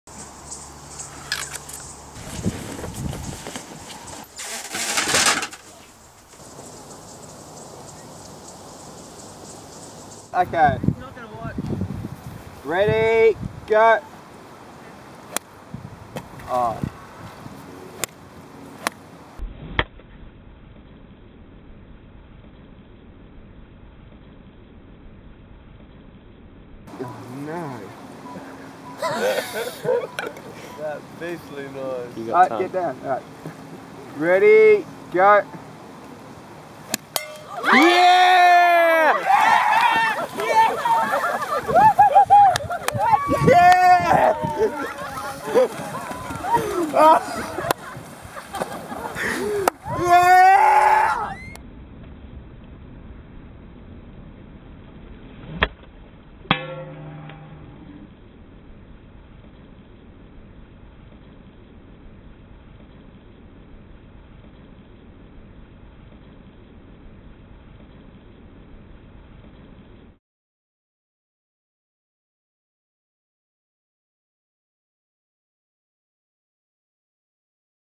Incredible Golf Trick Shot - How Ridiculous
Seriously ridiculous golf trick shot!

for more info on our passion to see children released from poverty.

While on holiday in Busselton, Western Australia we decided it would be a good time to try something different. We came up with the idea of hitting a dinner plate mid air with a golf ball on the beach.

For those of you wondering, yes we did pick up the broken ceramic plate pieces from the beach and we swam safely at the same location for the next few days without any problems. But thanks for your concern about safety, it's important!
Overall, we hope you enjoy the video as much as we did making it.